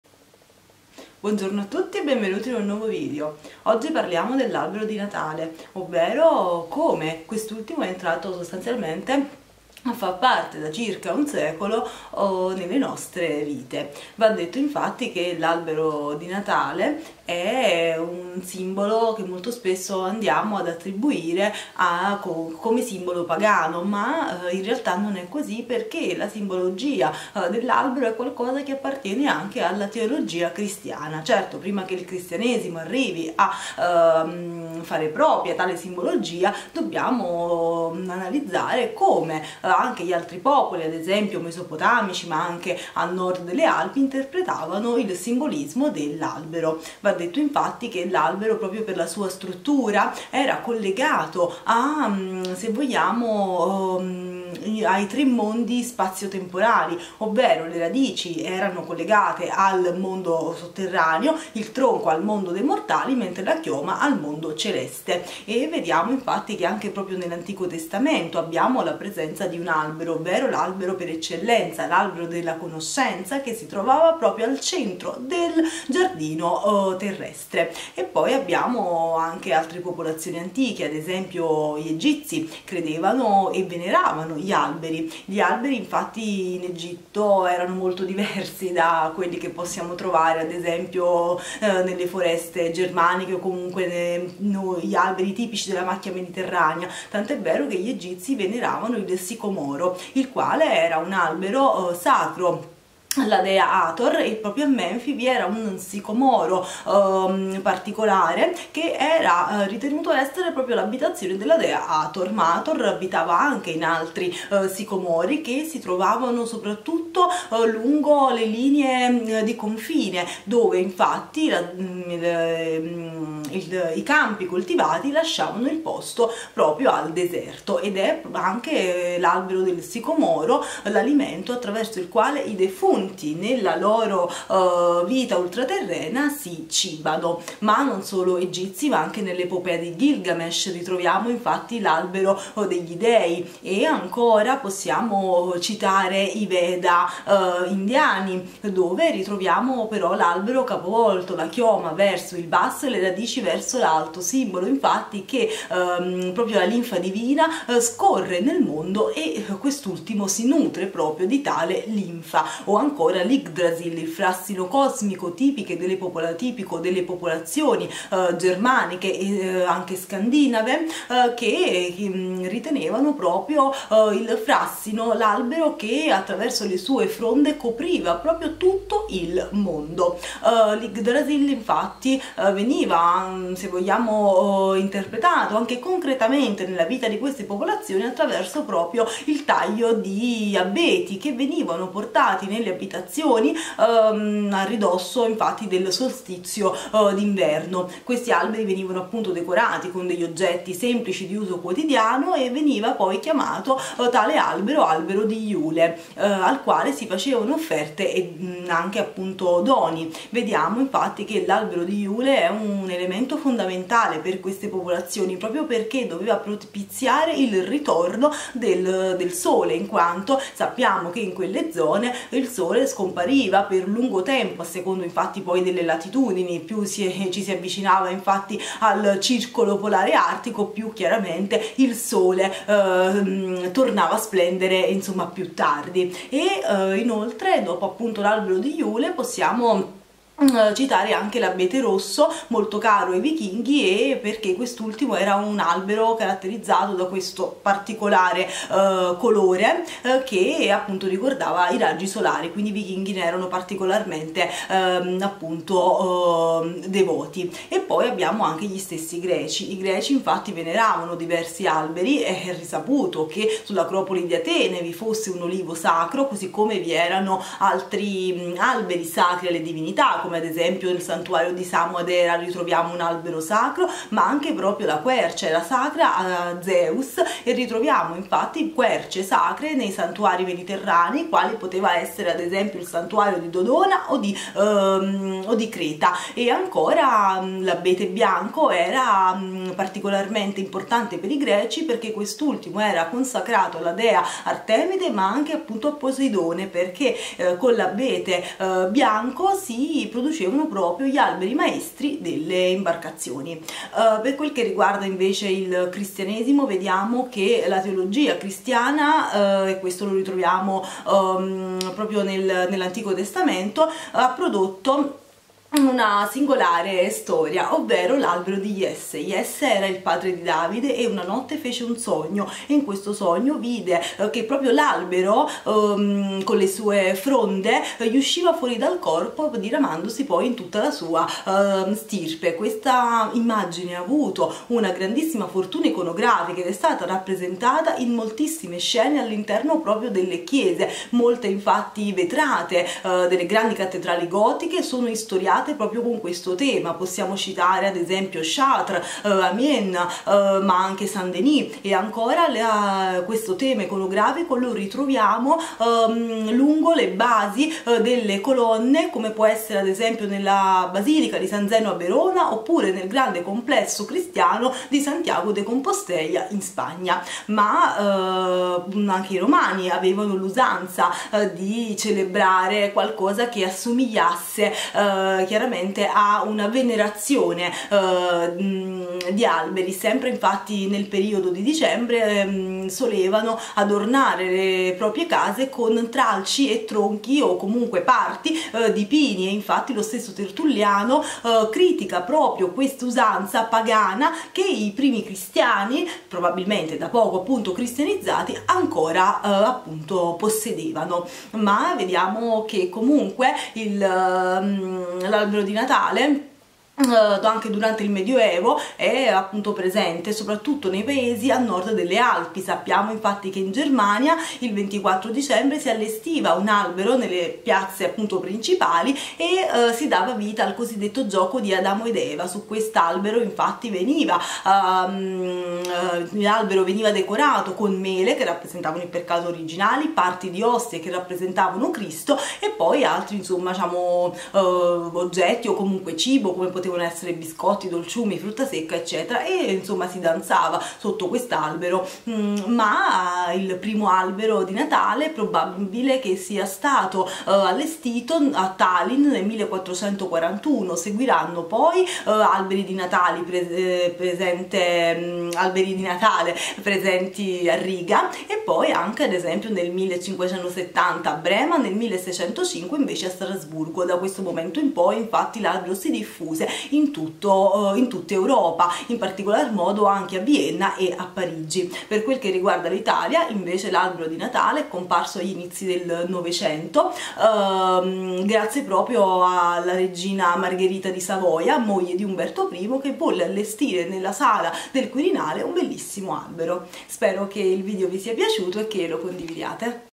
Perché facciamo l'albero di Natale?
Spesso riferendoci all'albero di Natale lo definiamo un simbolo o un'usanza appartenente al mondo pagano di cui però non possiamo fare più a meno, ebbene l'albero non ha a che fare con la cultura pagana. Esso, infatti, era riconosciuto universalmente come simbolo di vita e lo ritroviamo in molte culture: dagli Egizi ai Sumeri, passando per la Grecia fino ad arrivare alla Roma antica ed oltre le Alpi. Il cristianesimo assorbì tutta questa cultura arborea traferendola nell'iconografia cristiana dei primi secoli tanto è vero che ritroviamo alcune rappresentazioni della croce di Cristo dipinte come se fossero proprio degli alberi oppure le rappresentazioni dell'Albero di Iesse, racconto singolare di un sogno fatto dal padre del futuro re Davide.

Altri video sul Natale

Bibliografia di riferimento:
- A. Bertolini, La simbologia dell'albero nel linguaggio mitico-religioso, (saggio reperibile on-line).
- L. Tuan, Alberi magici, De Vecchi, Milano, 2023.
- J. Markale, Il Cristianesimo celtico e le sue sopravvivenze popolari, Edizioni Arkeios, Roma, 2014.